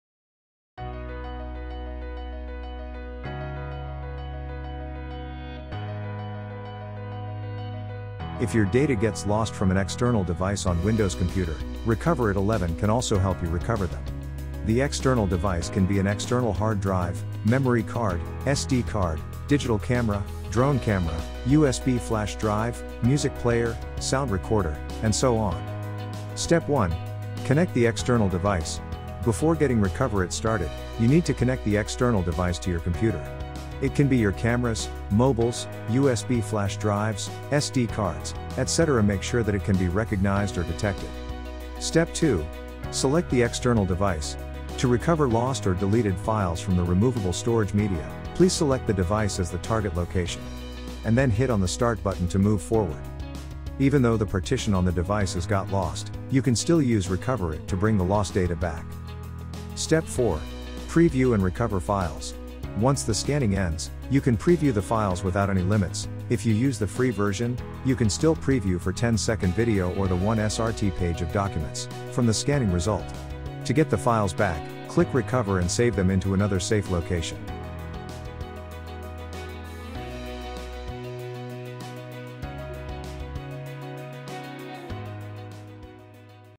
Guide—How to Recover Data from External Devices? (Windows)
If your data gets lost from an external device on Windows computer, Recoverit 12 can also help you recover them. In this video, we'll guide you through the process of recovering data from external devices. Learn more about the feature and download it

The external device can be an external hard drive, memory card, SD card, digital camera, Drone camera, USB flash drive, music player, sound recorder, and so on.